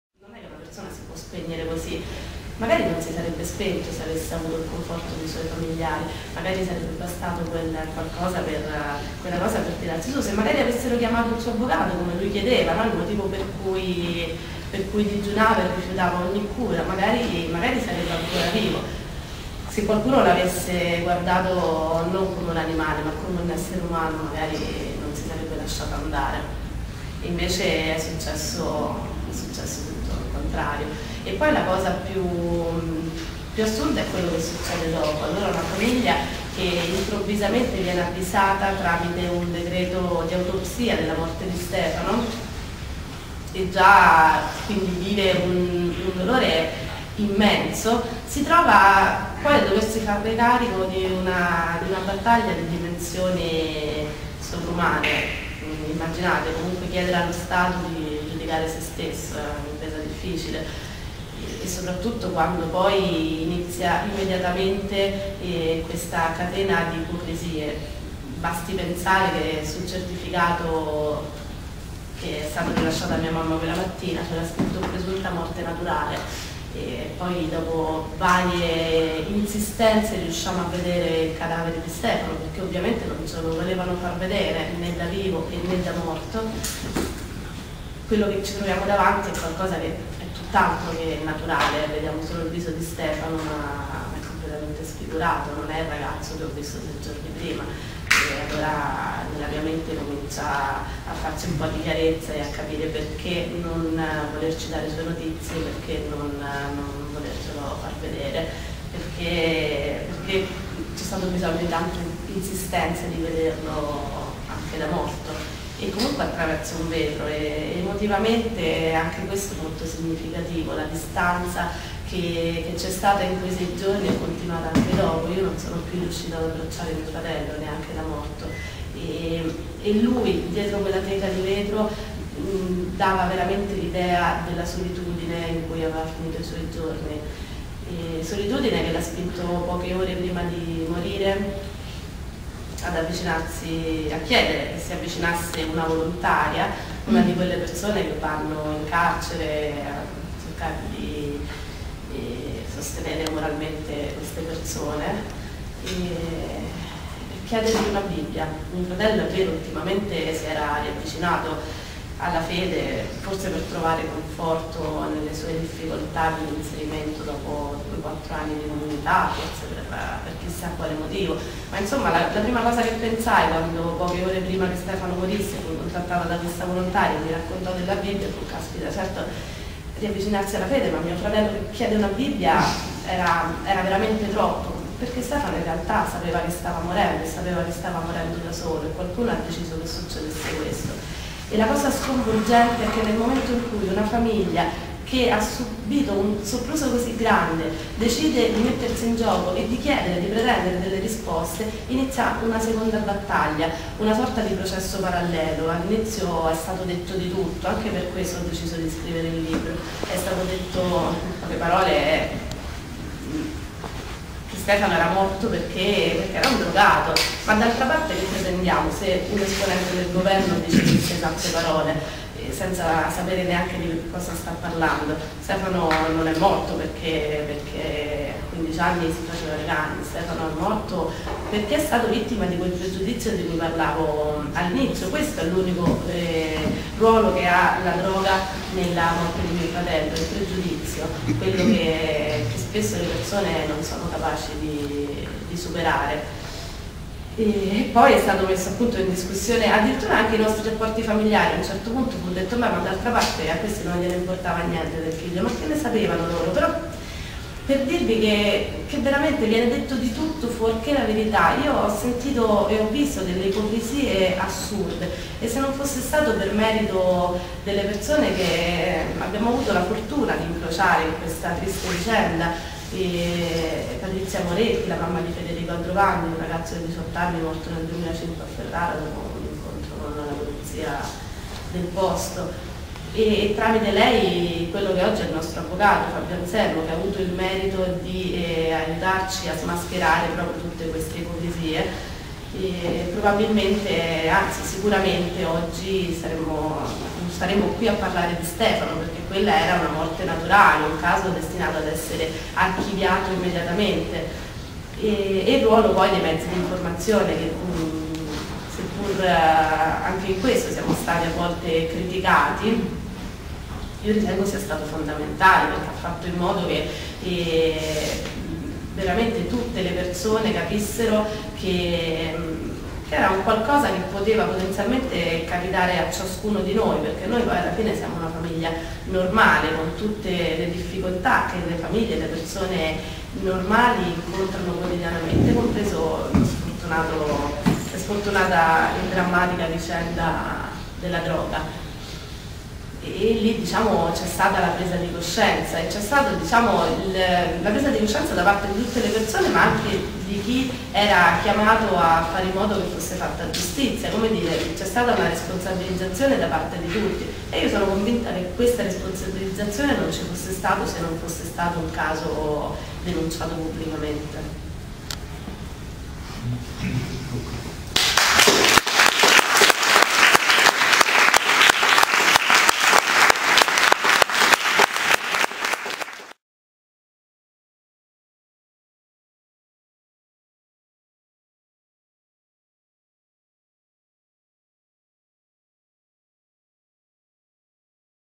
Università Tor Vergata. Presentazione del libro di Ilaria Cucchi 2/2
Ilaria Cucchi presenta il libro, "Vorrei dirti che non eri solo", che racconta della terribile morte del fratello Stefano, avvenuta nel carcere di Regina Coeli, il 22 ottobre 2009.
di Alessandra Vitullo